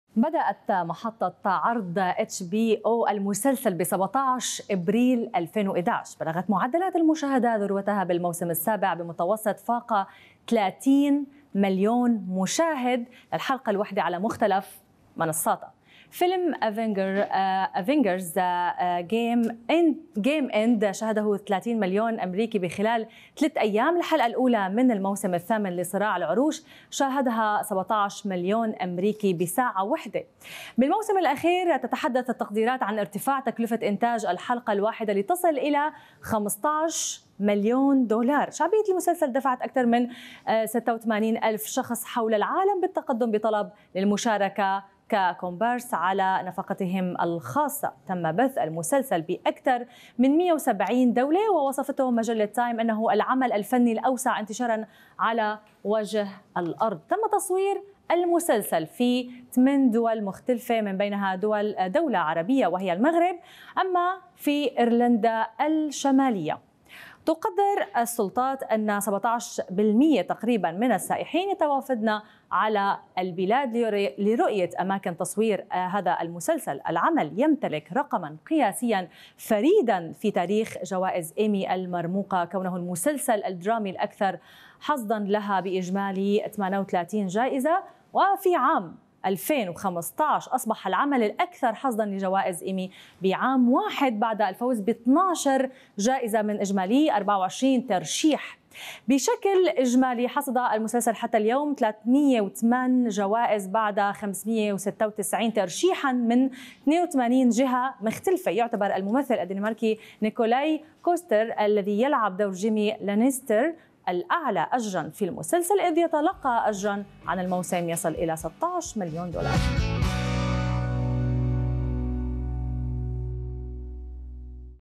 صراع العروش.. معلومات وحقائق
بدأت محطة HBO عرض المسلسل في 17 أبريل 2011
بلغت معدلات المشاهدة ذروتها بالموسم السابع بمتوسط فاق 30 مليون مشاهد للحلقة الواحدة على مختلف منصاتها
فيلم أفنجرز end game شاهده ٣٠ مليون أميركي في خلال ٣ أيام .. الحلقة الأولى من الموسم الثامن لصراع العروش شاهدها ١٧ مليون أميركي في ساعة واحدة
في الموسم الأخير تتحدث التقديرات عن ارتفاع تكلفة إنتاج الحلقة الواحدة لتصل إلى 15 مليون دولار
شعبية المسلسل دفعت أكثر من 86 ألف شخص حول العالم بالتقدم بطلب للمشاركة كـ "كومبارس" على نفقتهم الخاصة
تم بث المسلسل في أكثر من 170 دولة ووصفته مجلة تايم أنه العمل الفني الأوسع انتشارا على وجه الأرض
تم تصوير المسلسل في 8 دول مختلفة من بينها دولة عربية وهي المغرب
في إيرلندا الشمالية
تقدر السلطات أن 17% تقريبا من السائحين يتوافدن على البلاد لرؤية أماكن تصوير المسلسل
العمل يمتلك رقما قياسيا فريدا في تاريخ جوائز "إيمي" المرموقة كونه المسلسل الدرامي الأكثر حصدا لها بإجمالي 38 جائزة
في عام 2015 أصبح العمل الأكثر حصدا لجوائز إيمي في عام واحد بعد الفوز بـ 12 جائزة من إجمالي 24 ترشيح
بشكل إجمالي حصد المسلسل حتى اليوم 308 جوائز بعد 596 ترشيحا من 82 جهة مختلفة
يعتبر الممثل الدنماركي نيكولاي كوستر الذي يلعب دور جيمي لانيستر الأعلى أجرًا في المسلسل، إذ يتلقى أجرًا عن الموسم يصل إلى 16 مليون دولار